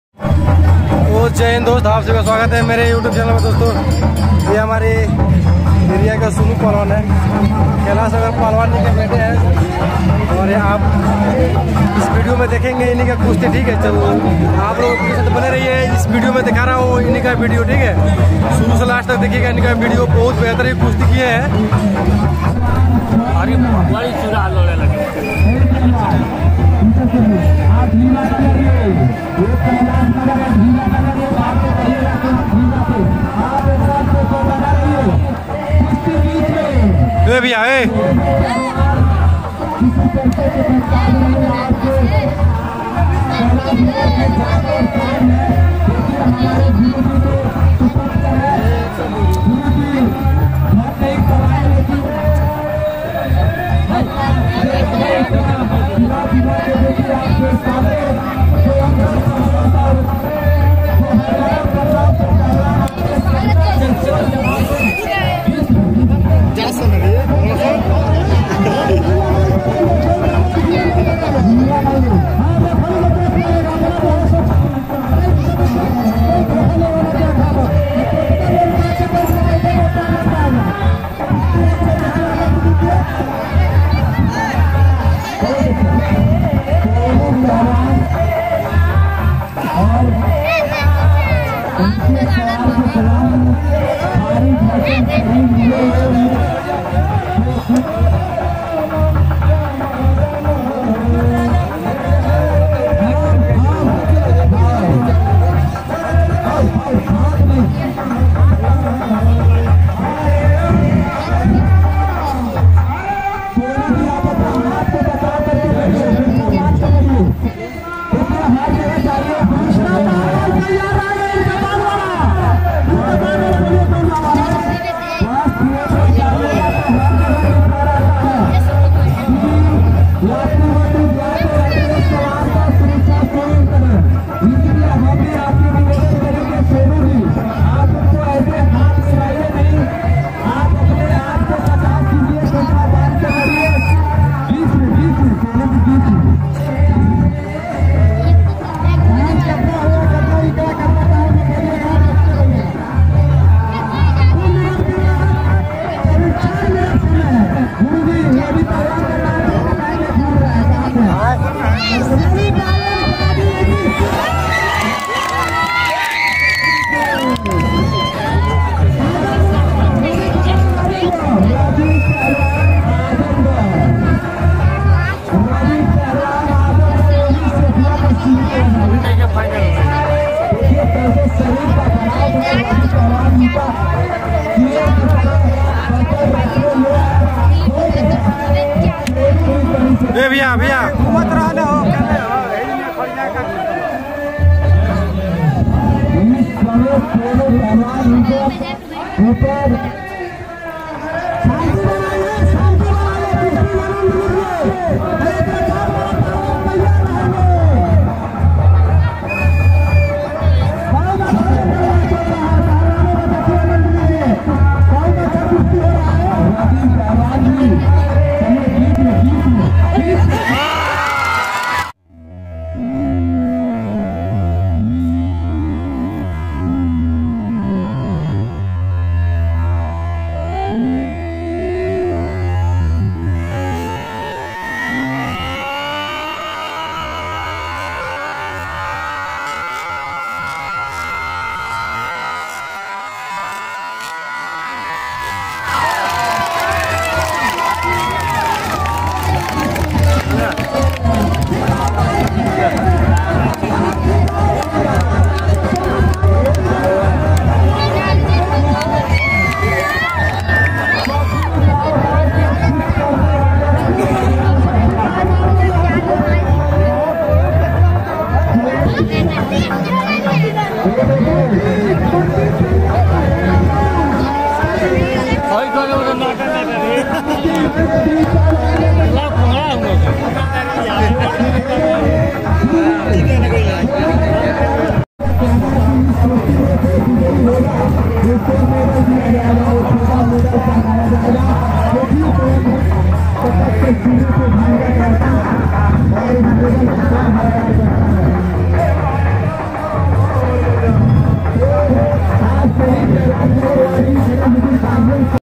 || सोनू पहलवान || 🤼चम्पारण के शान छोटेलाल पहलवान के लाल आज फिर किया कमाल // kusti baaj //
desi dangal
Desi pahalwan